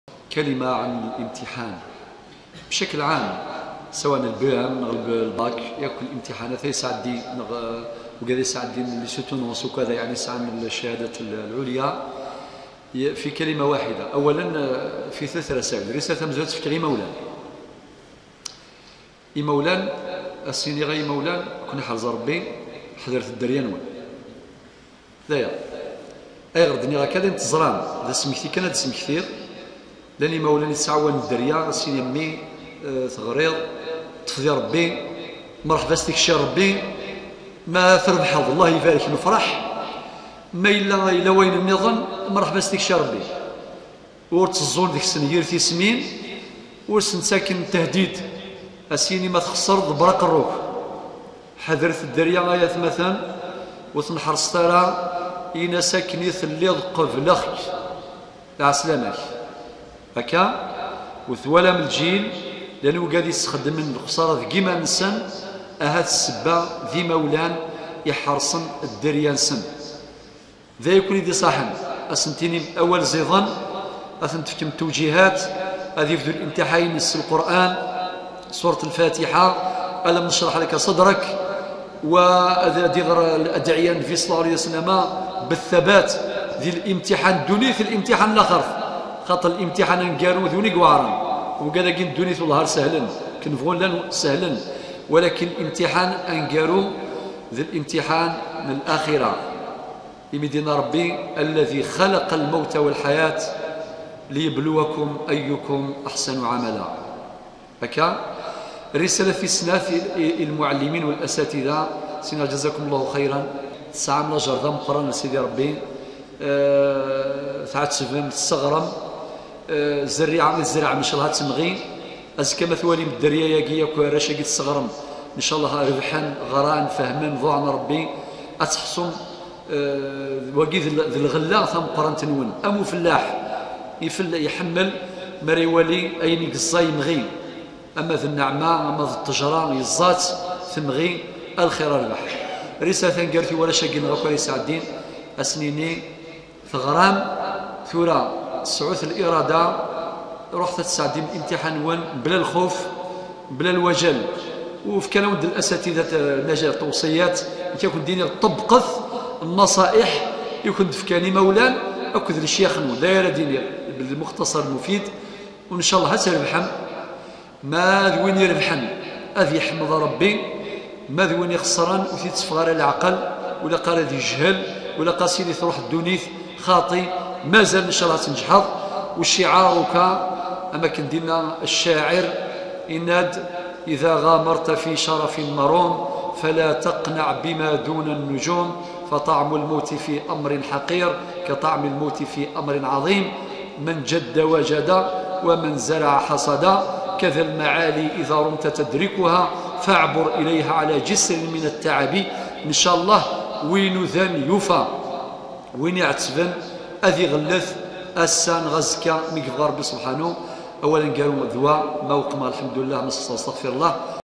Cheikh Said Bouizeri - Kra n tebratin i warraw nnaɣ qbel ikayaden - رسائل لأبنائنا قبل الامتحانات
الدكتور سعيد بويزري - Kra n tebratin i warraw nnaɣ qbel ikayaden - رسائل لأبنائنا قبل الامتحانات
مسجد الشيخ الحداد عزازقة تيزي وزو الجزائر - 05 جوان 2014